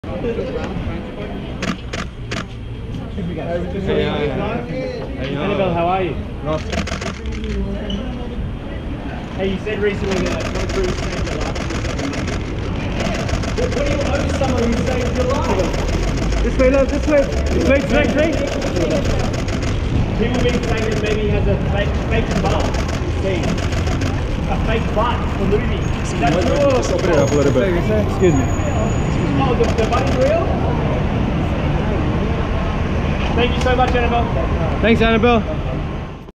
Annabelle Wallis Says Co-Star Tom Cruise's Butt 'Is Real' In 'The Mummy'
Wednesday, September 27, 2017 - "The Mummy" star Annabelle Wallis arrives in L.A. after attending Paris Fashion Week, and when asked about rumors that co-star, Tom Cruise's butt is fake in the film, Wallis shoots the rumors down, saying, "it is real!" Also see what she has to say about Cruise "saving" her life.